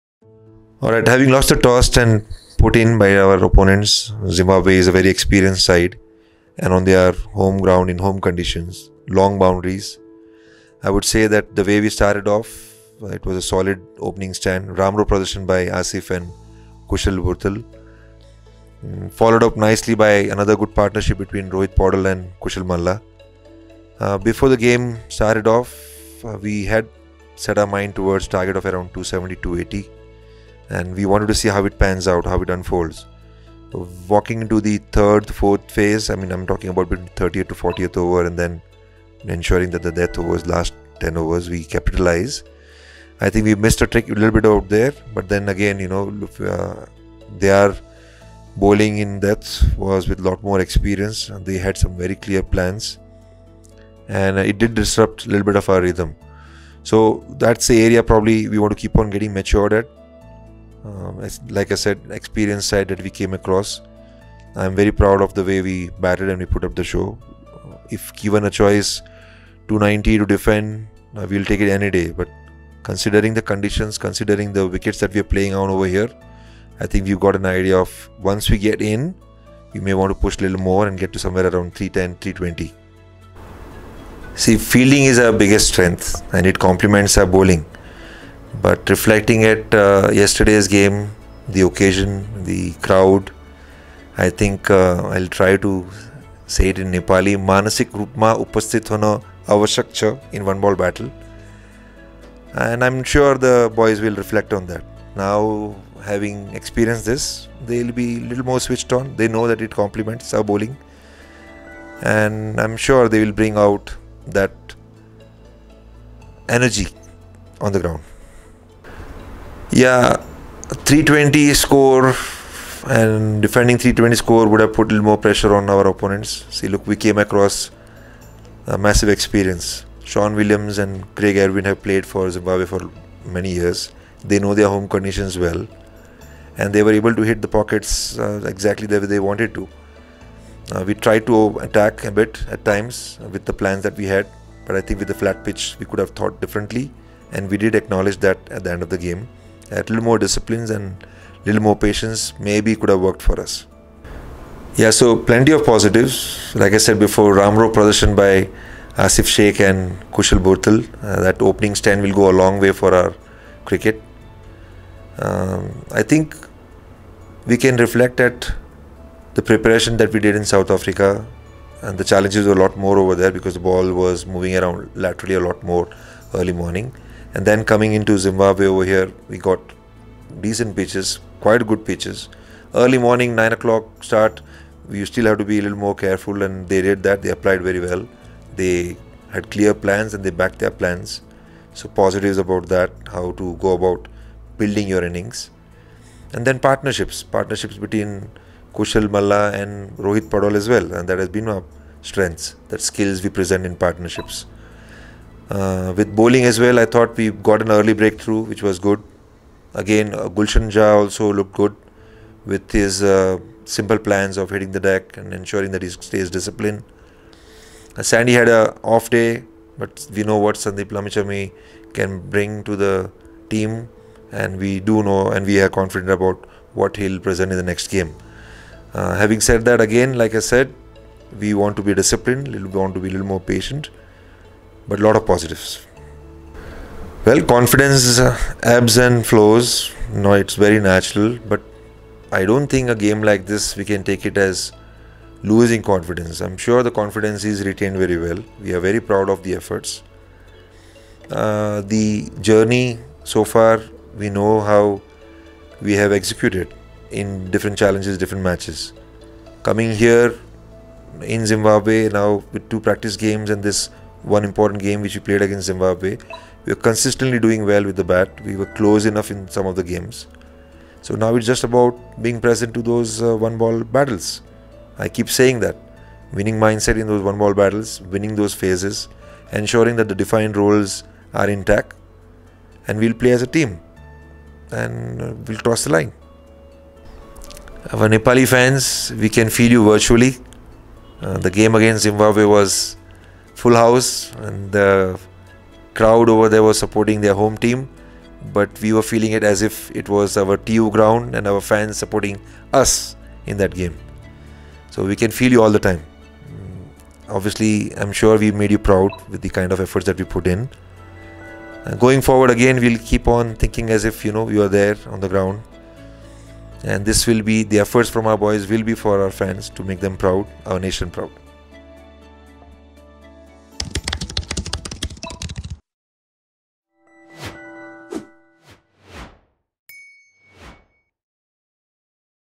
Nepal vs Zimbabwe ICC Qualifier: Monty Desai's Exclusive Insights | Head Coach Interview
Join us for an exclusive interview with Monty Desai, Head Coach of the Nepali Cricket Team, as he shares insights on their first match in the ICC Qualifier against Zimbabwe. Coach Desai discusses Nepal's strengths, learning experiences, and future approaches. Get a glimpse into the team's preparation, player performances, and strategic decisions. Don't miss this must-watch interview for cricket enthusiasts and Nepali cricket fans.